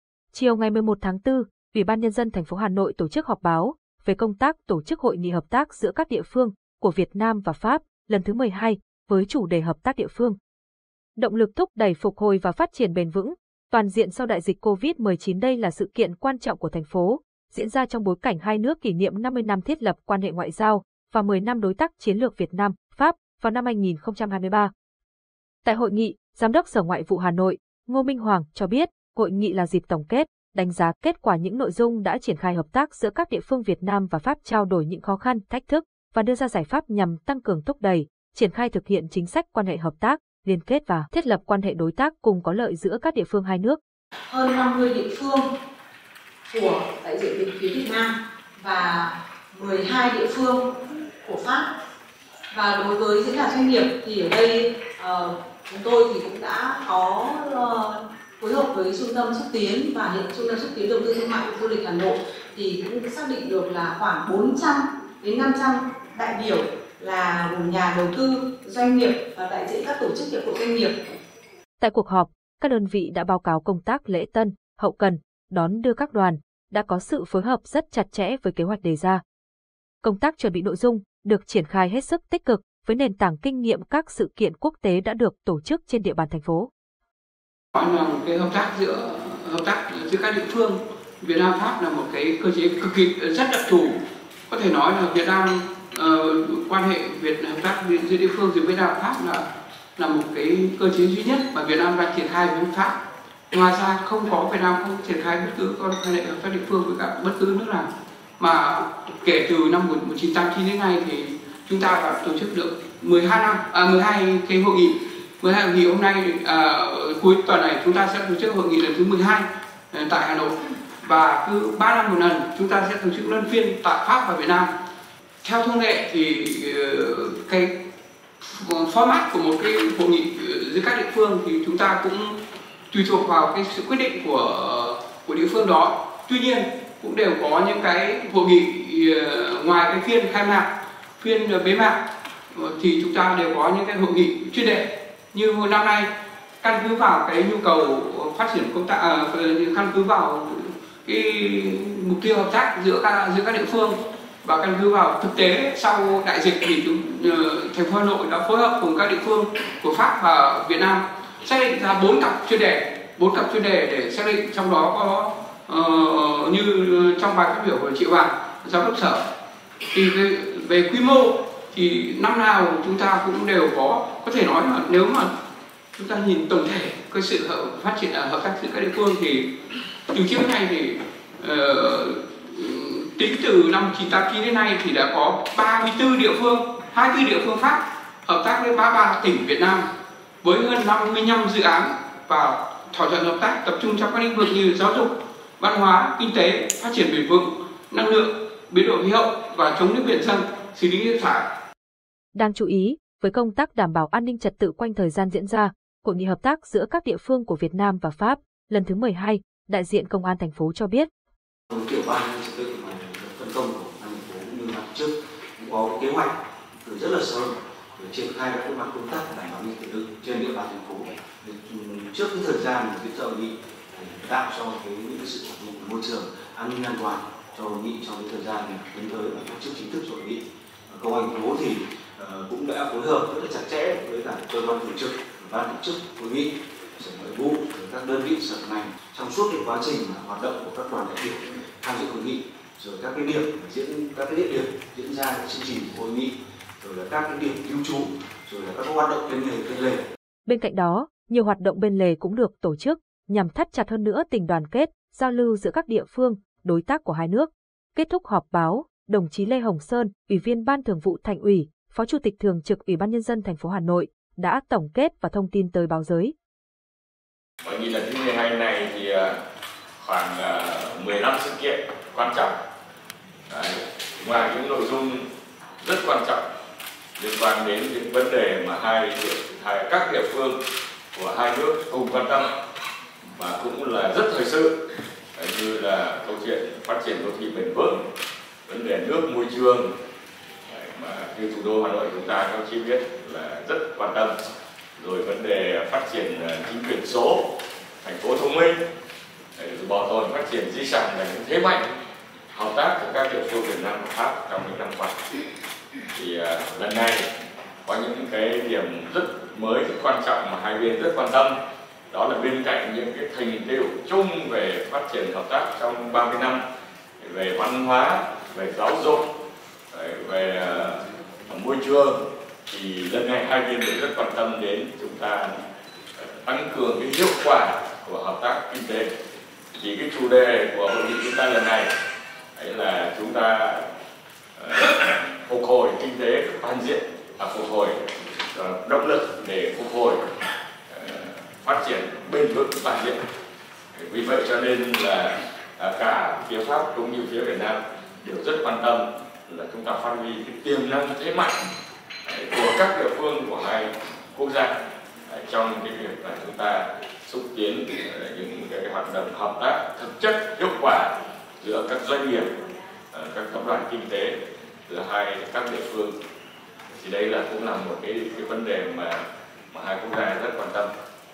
Hà Nội sẵn sàng cho Hội nghị hợp tác giữa các địa phương Việt Nam - Pháp lần thứ 12 | TTTĐ
Chiều 11/4, Ủy ban Nhân dân thành phố Hà Nội tổ chức họp báo về công tác tổ chức Hội nghị hợp tác giữa các địa phương của Việt Nam và Pháp lần thứ 12.